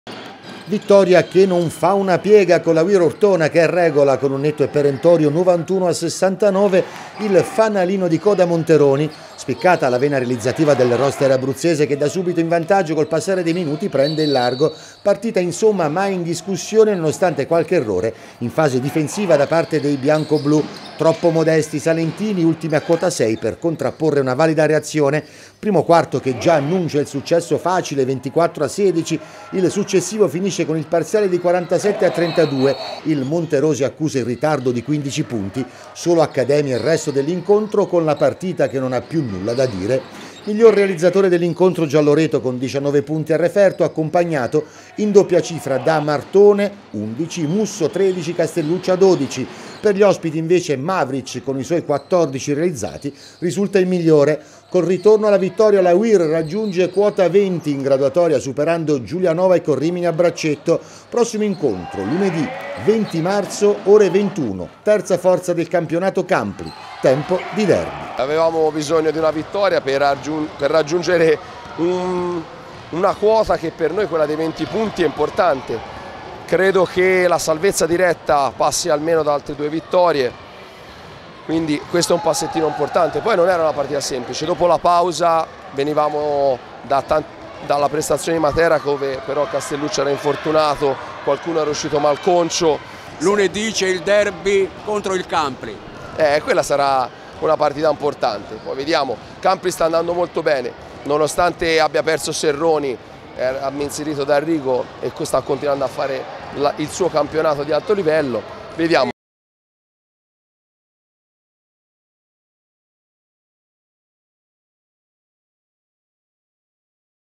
Basket: Ortona - Monteroni 91-69
Ritorno in patria vittorioso per la We're che senza troppa fatica ha la meglio sul fanalino di coda Monteroni con il punteggio di 91-69.